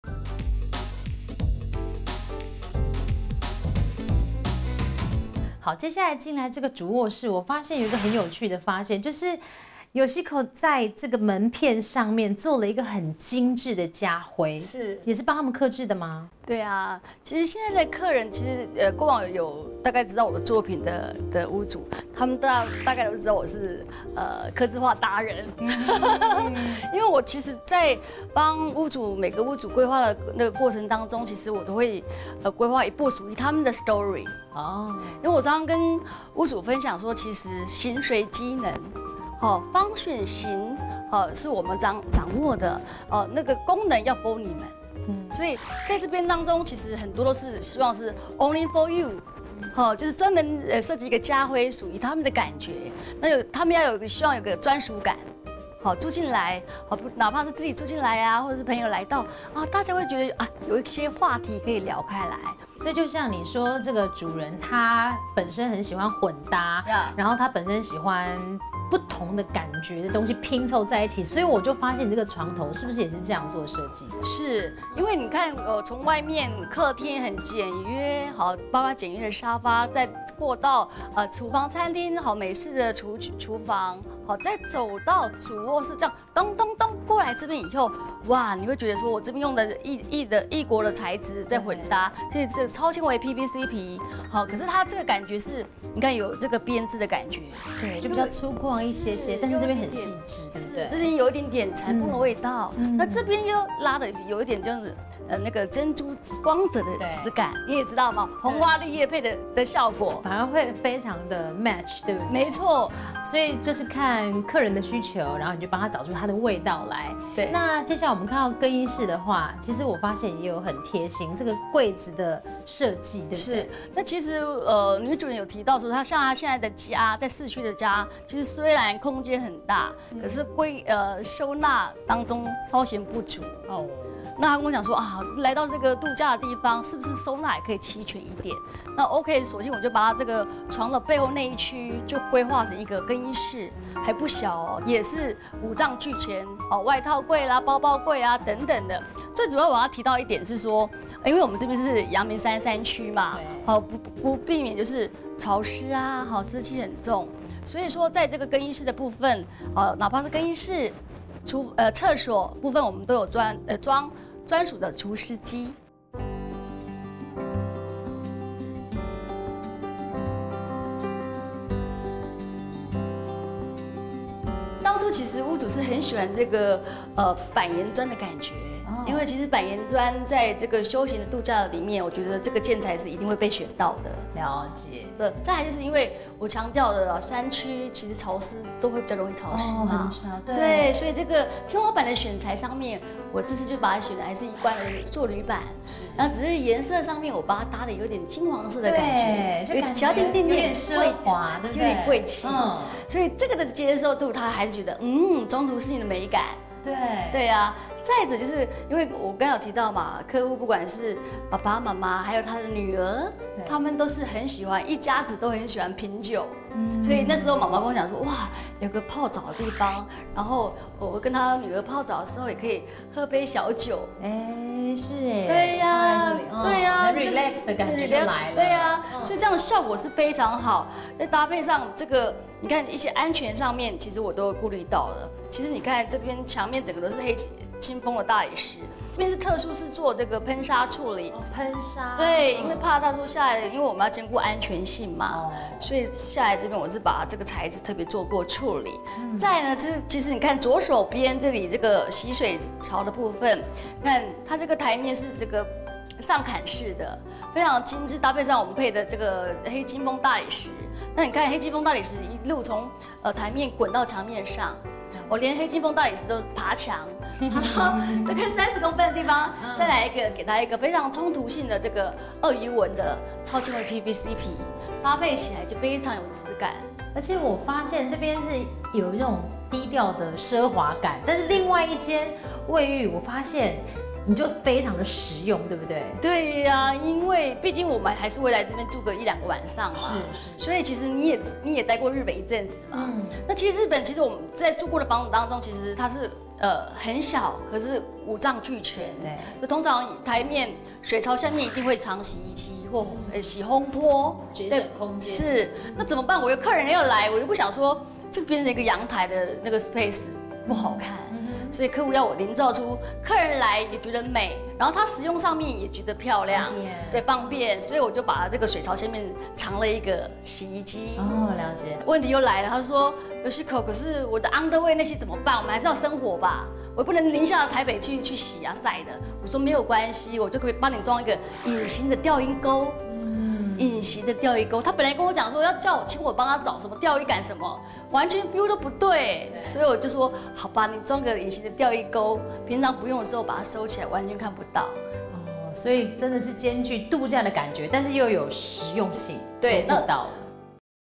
異國混搭 自然休閒渡假屋《主臥／衛浴》【程翊設計聯盟-詹芳玫】
相關影片請至王牌設計師網站皮革編織混搭珍珠光澤繃布的設計營造主臥室奢華感。自然質樸的板岩磚搭配金華奢華的塑鋁板天花，混搭出主衛浴衝突的美感。